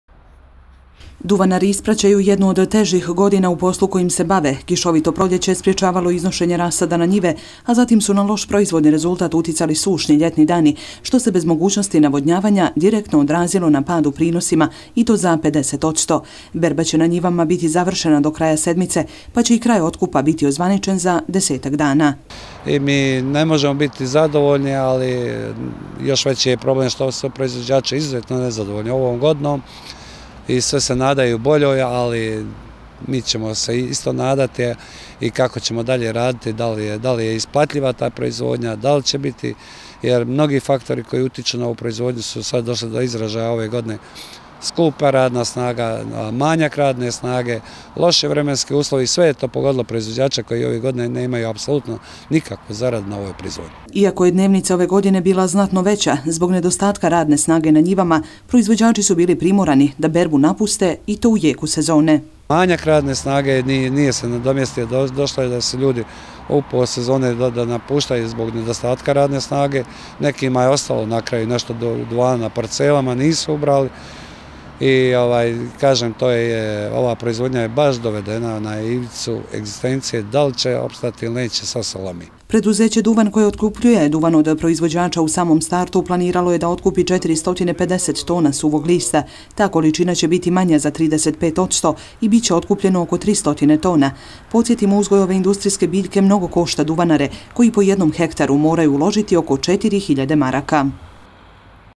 Otkup duvana
Ove godine prepolovljeni su prinosi duvana, nisu proizvođači ostvarili ni neophodan kvalitet lista, a sve se odrazilo i na pad otkupa. Nešto bolje rezultate postigli su na njivama proizvođači koji su u sadnju ušli nešto kasnije. Ipak, prosječan kvalitet otkupljenog duvana je ispod prosjeka, što duvanarima ne garantuje cijenu koja bi pokrila troškove proizvodnje.